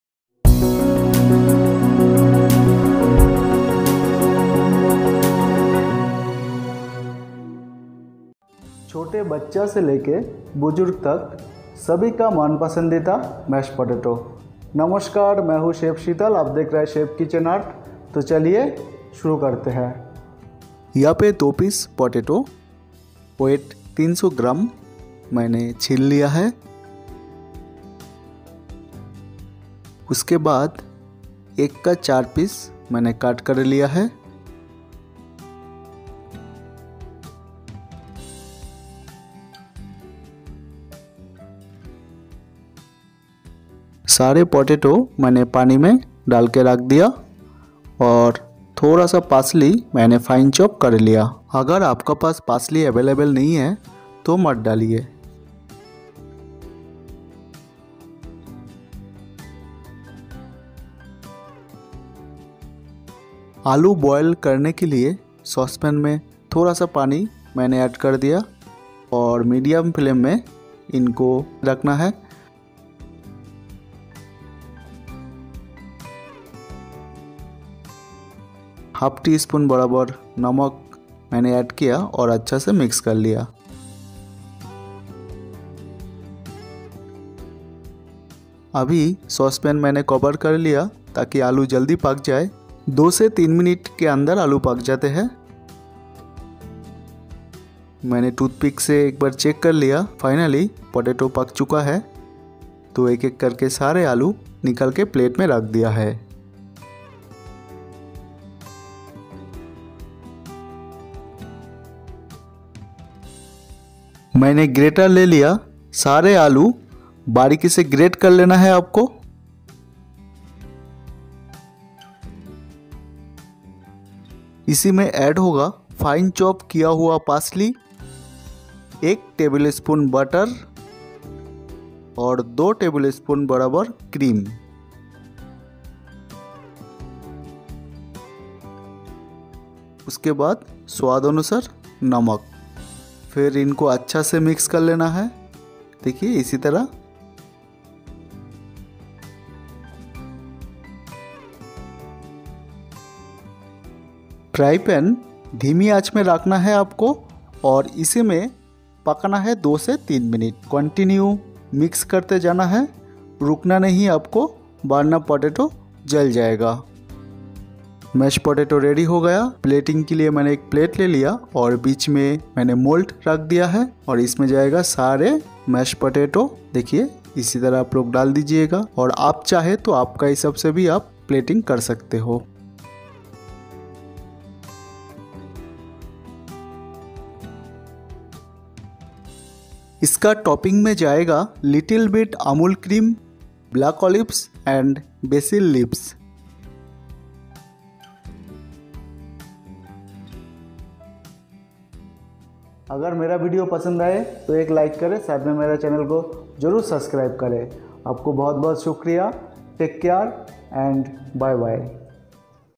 Mashed Potato Recipe | Restaurant Style Mashed Potatoes Recipe | Kids Recipes
Mashed Potato Recipe | Restaurant Style Mashed Potatoes Recipe | Kids Recipes | Chef Kitchen Art

Mashed potatoes is a comforting and unique dish that is smooth and fluffy. A basic mashed potato recipe is just made with potatoes, cream , butter, salt and parsley . But You can build upon this basic recipe by adding various herbs, greens, seasonings and spices. Sky is the limit.

Tips- 1) Do use seive (channi) to remove any lumps in the boiled potatoes.
2) Use high starch potatoes (old potatoes).

MASHED POTATOES INGREDIENTS :
- 300 gm potato
- salt to taste
- 1tsp parsley chopped
- 1 tbsp butter
- 2 tbsp cream
Garnishing
- 1/2 tsp cream
- 2 pieces slice of black olives
- 1 pieces basil leaves